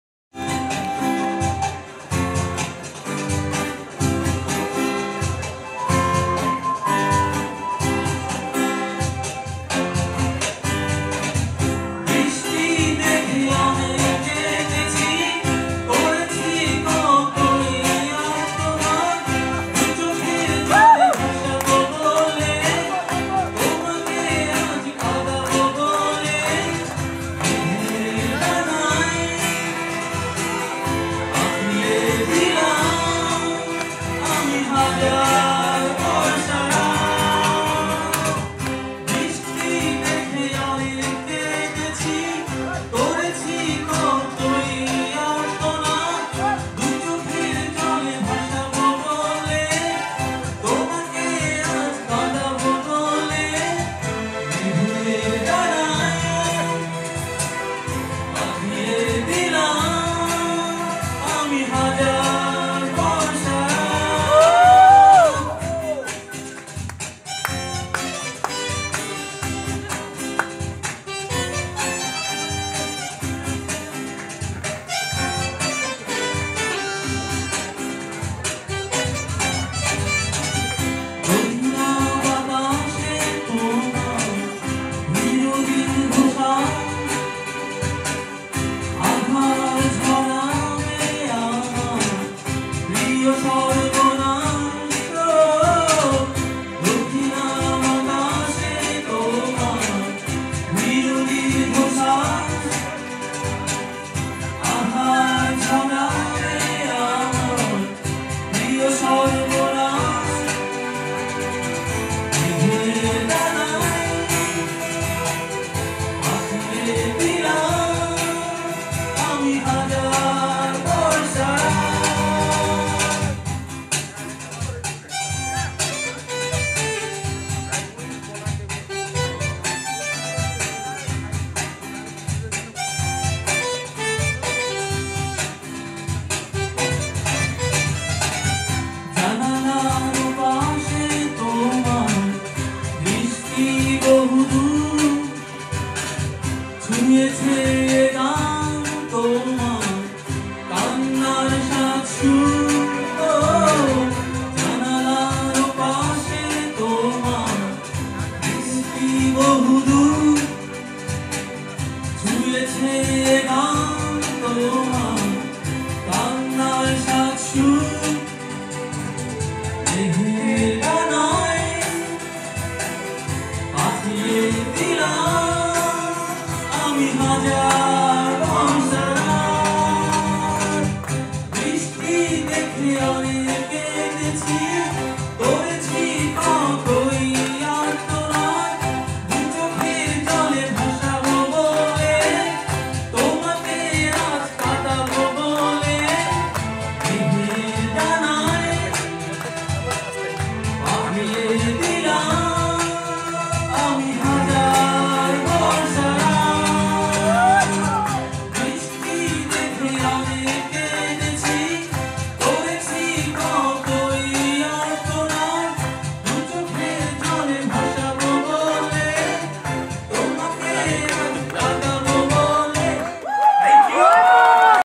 Band song
Sir John Wilson School Annual Staff party 💃 at Gulshan Club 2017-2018